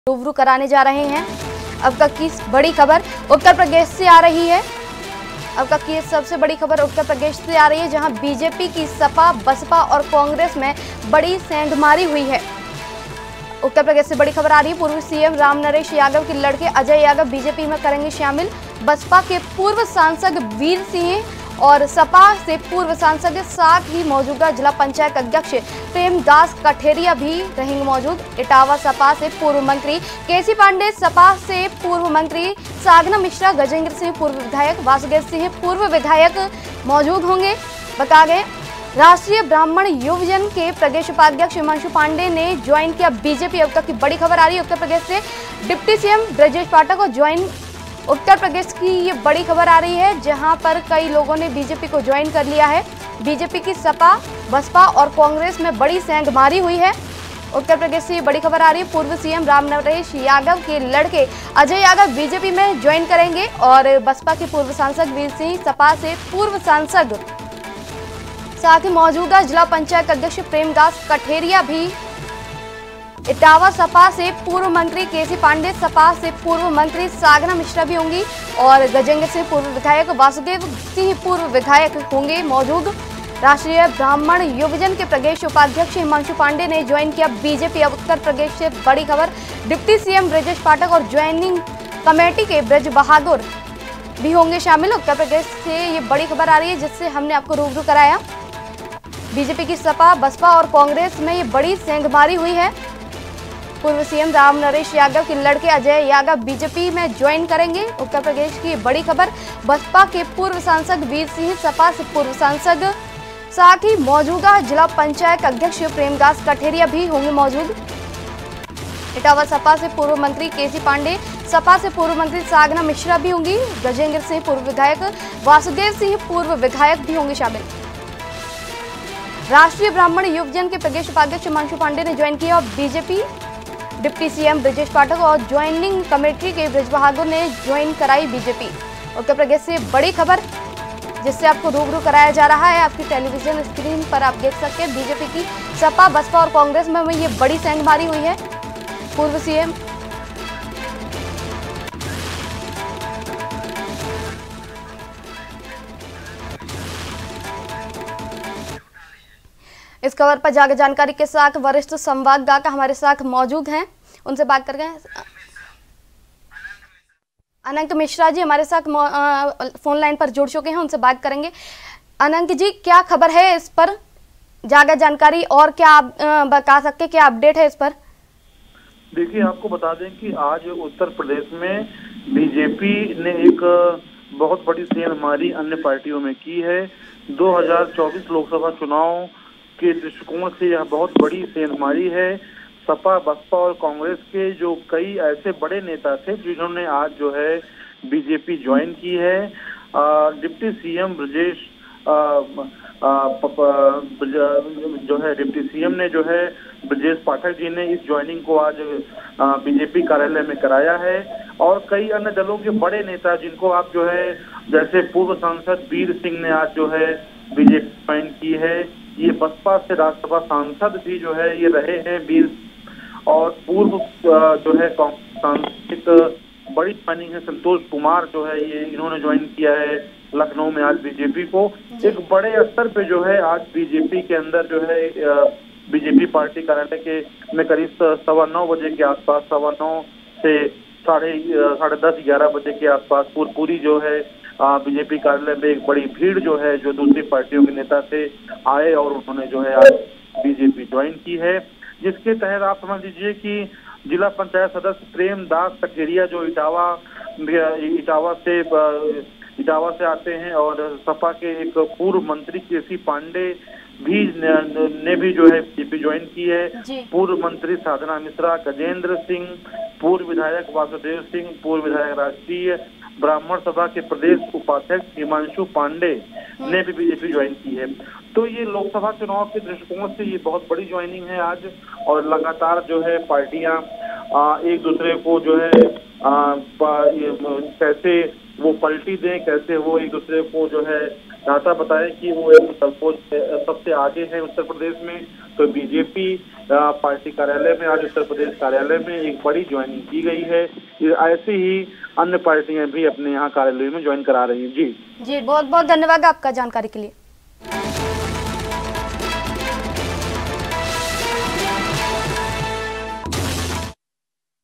बीजेपी की सपा, बसपा और कांग्रेस में बड़ी सेंधमारी | ANB NEWS
उत्तर प्रदेश से बड़ी ख़बर
बीजेपी की सपा, बसपा और कांग्रेस में बड़ी सेंधमारी
पूर्व सीएम रामनरेश यादव के लड़के अजय यादव बीजेपी करेंगें ज्वाईन
बसपा के पूर्व सांसद वीर सिंह, सपा से पूर्व सांसद
साथ ही मौजूदा जिला पंचायत अध्यक्ष प्रेम दास कठेरिया
इटावा सपा से पूर्व मंत्री केसी पाण्डेय, सपा से पूर्व मंत्री साधना मिश्रा
गजेंद्र सिंह पूर्व विधायक, वासुदेव सिंह पूर्व विधायक
राष्ट्रीय ब्राह्मण युवजन के प्रदेश उपाध्यक्ष हिमांशु पांडेय ने ज्वॉइन की बीजेपी
डिप्टी सीएम बृजेश पाठक और ज्वाइनिंग कमेटी के बृजबहादुर ने ज्वॉइन कराई बीजेपी